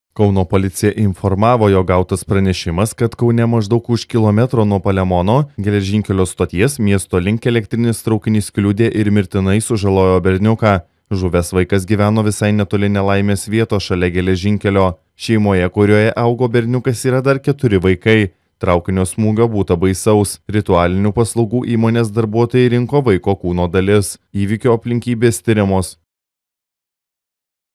Traukinys mirtinai sužalojo berniuką
Kauno policija informavo, 17 val. 44 min. gautas pranešimas, kad Kaune, maždaug už kilometro nuo Palemono geležinkelio stoties miesto link elektrinis traukinys kliudė ir mirtinai sužalojo berniuką. Žuvęs vaikas gyveno visai netoli nelaimės vietos, šalia geležinkelio. Šeimoje, kurioje augo berniukas, yra dar 4 vaikai. Traukinio smūgio būta baisaus, Ritualinių paslaugų įmonės darbuotojai rinko vaiko kūno dalis.